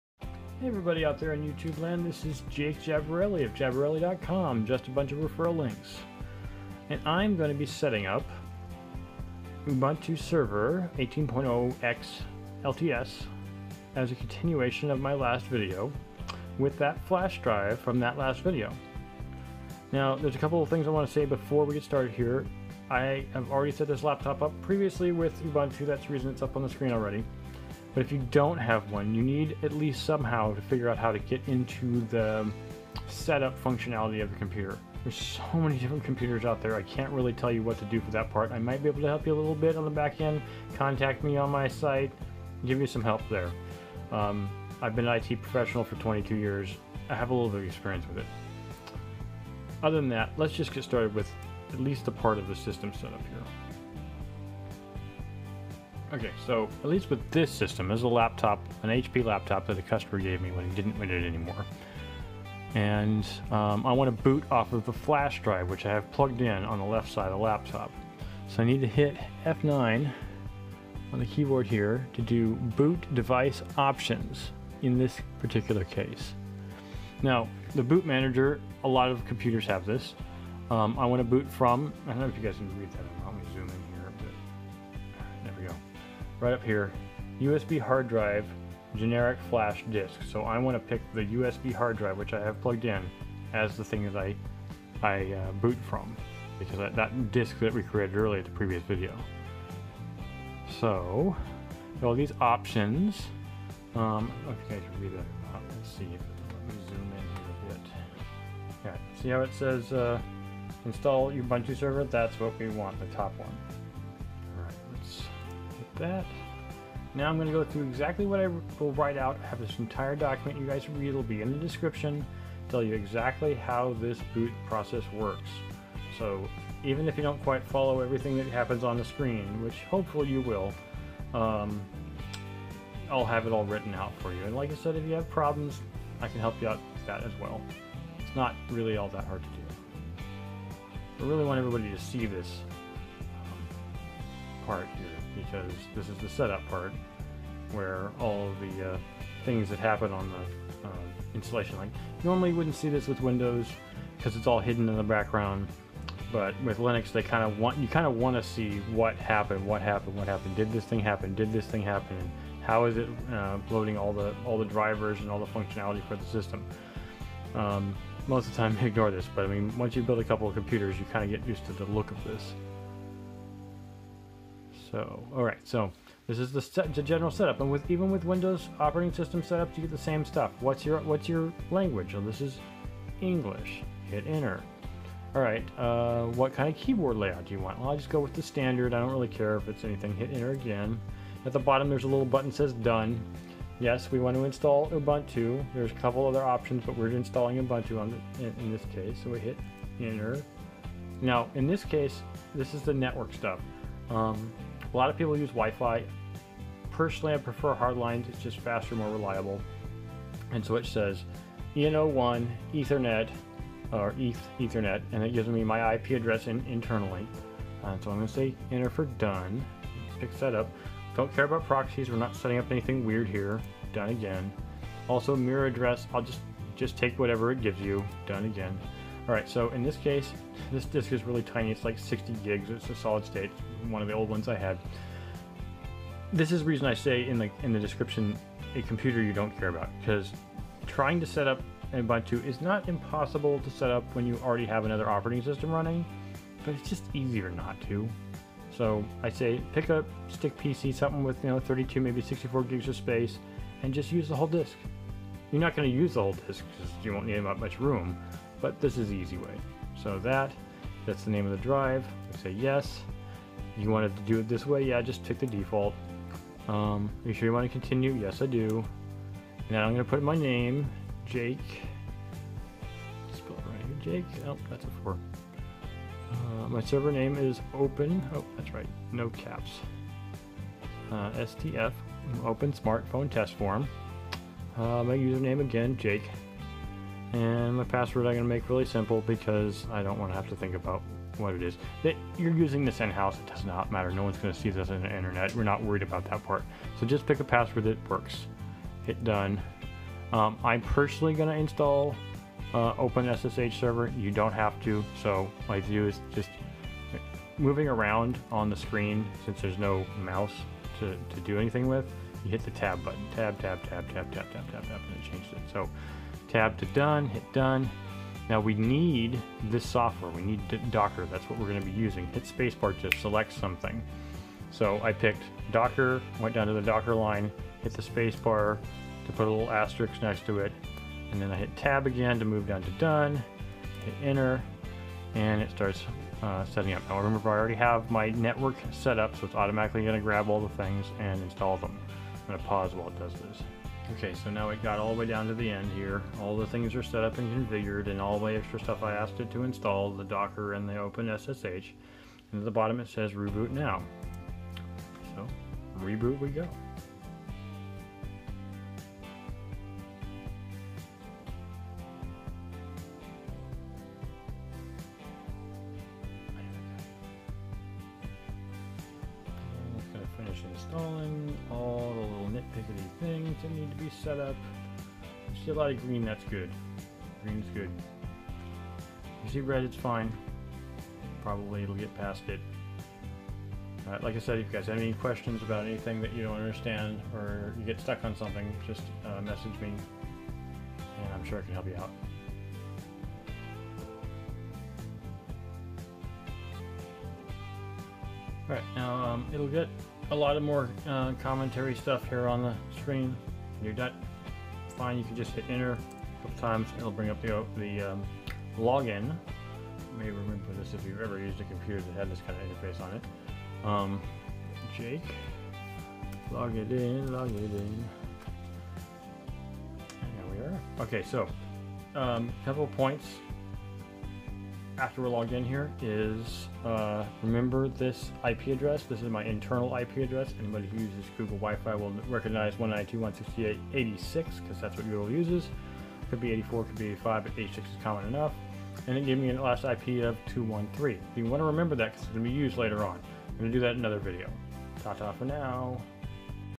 [OpenSTF - Part 2] Install Ubuntu
Here's how to INSTALL Ubuntu Server on your blank PC for OpenSTF

Series on How to Set Up OpenSTF

4. Using OpenSTF
...coming soon
Parts
Your Own Laptop / Mini PC
Stick PC

USB Stick Drive
32 GB